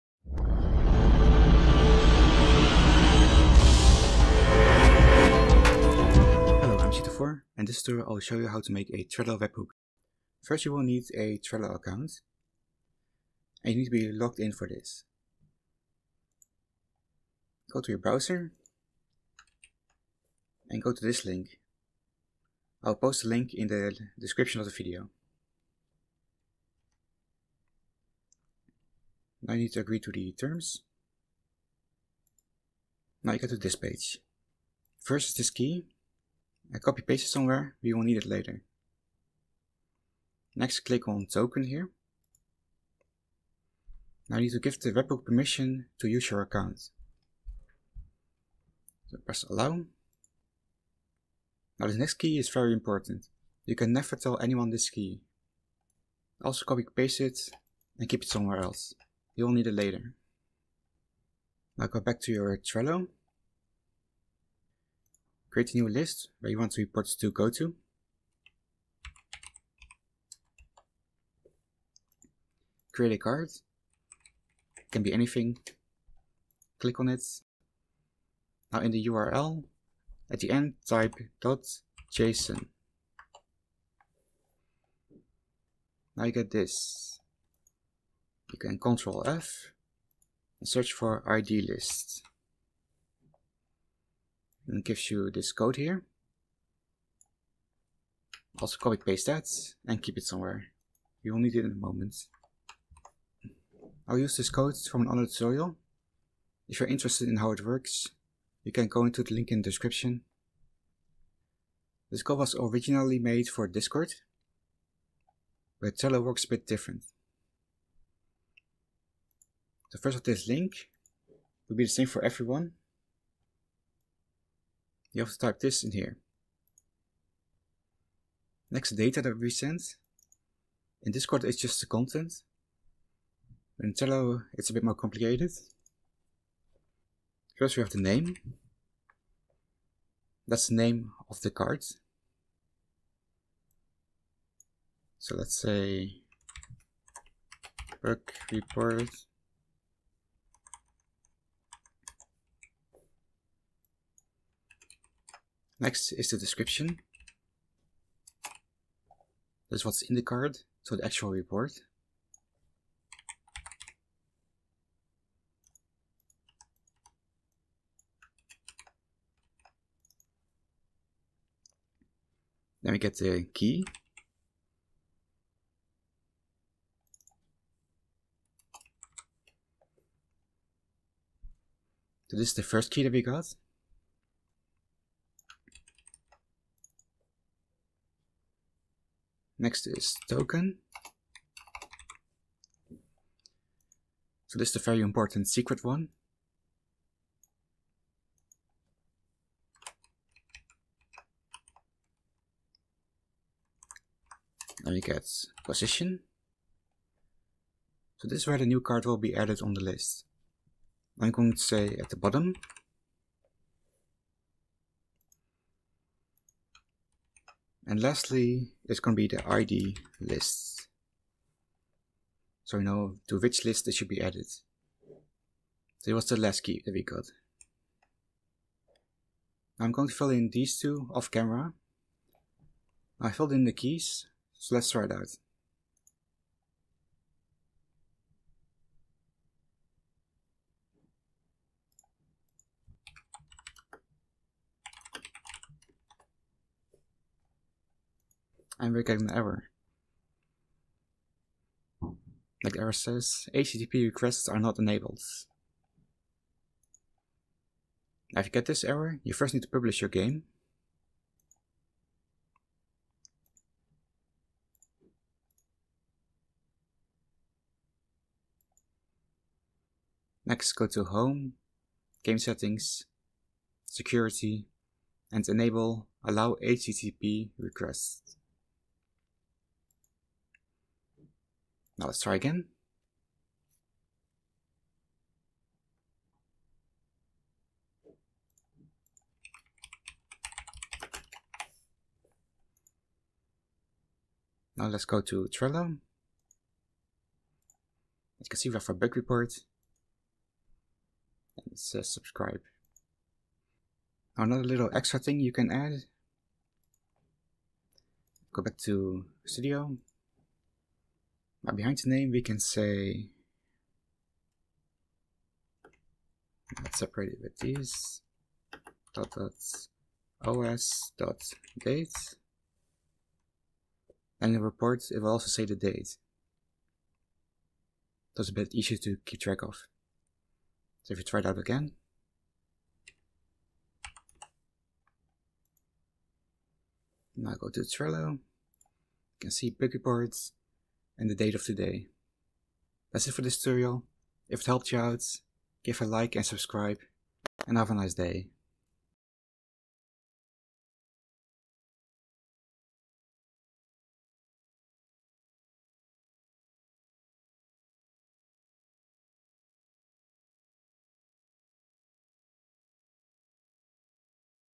Create A Trello Webhook
Learn how to create a trello webhook!
Since discord banned roblox from using their webhook trello is a great alternative.